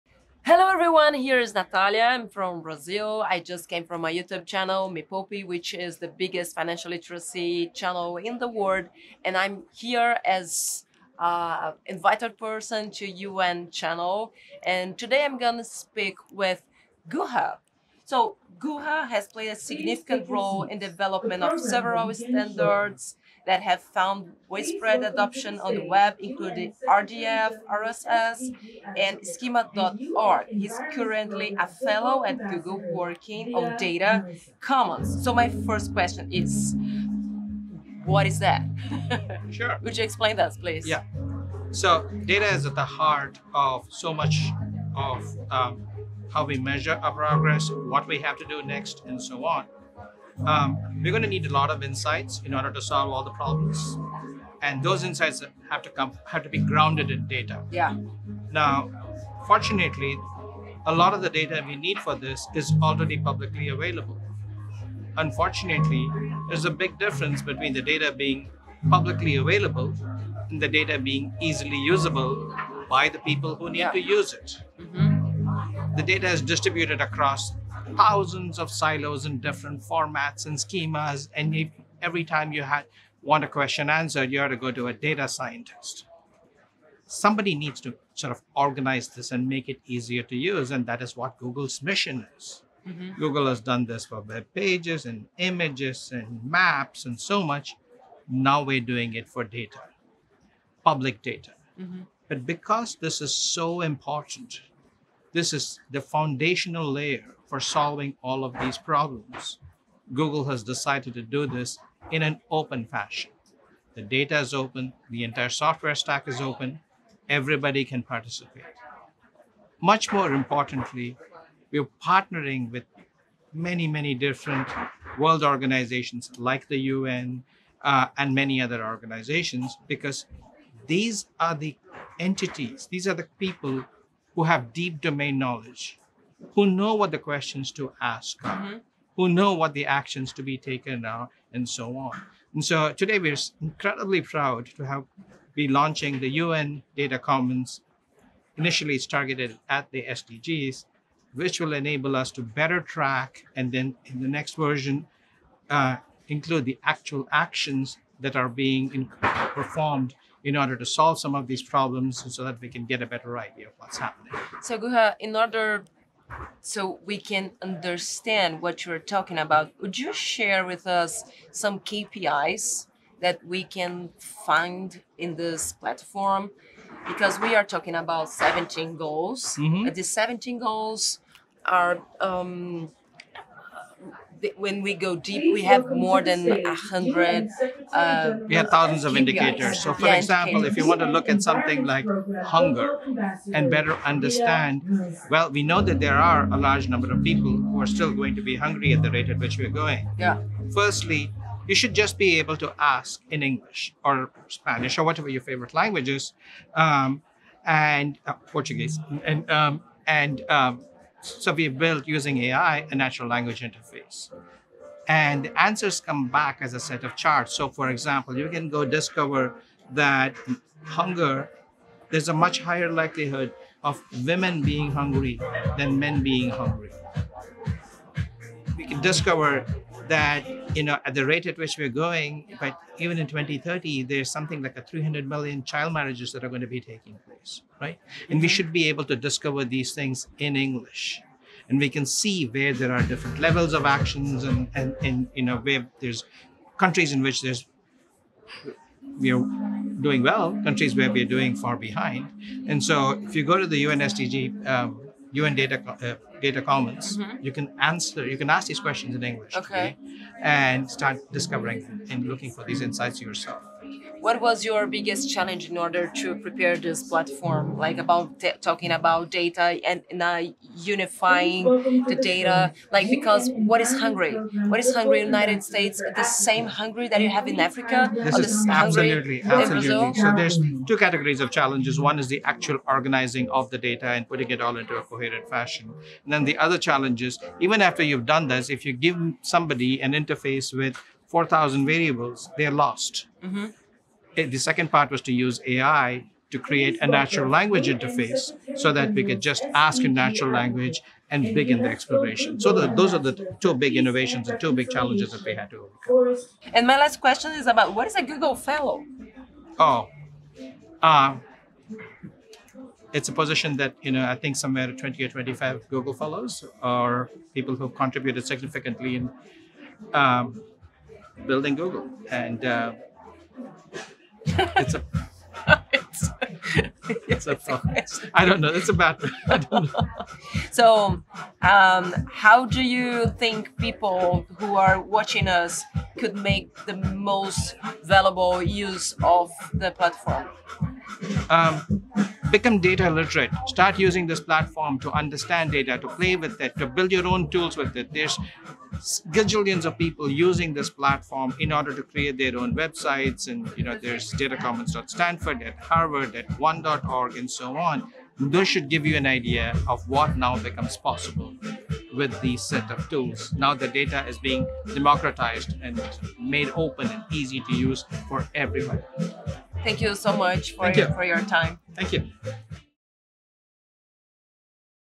Nathalia Arcuri meets R.V. Guha | Measuring Data on SDGs | Creator Studio at SDG Pavilion | UNGA 78
The SDG Pavilion is a convening space and art installation, designed by renowned artist Es Devlin at UN Headquarters in New York during high-level week of the UN General Assembly. The 78th Session of UNGA falls at the halfway mark to the 2030 deadline on the Sustainable Development Goals. The SDG Pavilion hosted a series of ‘Halftime Talks’ – stimulating sessions focused on unlocking the deep transitions needed to change the game in the second half and accelerate progress across the Goals.

The United Nations and non-profit agency Project Everyone partnered with global influencers to bring the message of the SDGs at Halftime to people across the world, through the Creator Studio at the SDG Pavilion – and inspire everyone to ImagineWinning.

Watch Nathalia Arcuri, founder of the world’s largest financial entertainment platform, Me Poupe!, speak with R.V. Guha, Google Fellow, about the importance of data for measuring and accelerating progress on the SDGs.

Host: Nathalia Arcuri @MePoupe @nathaliaarcuri
Channel: @Mepoupenaweb

Guest: R.V. Guha, Google Fellow, Google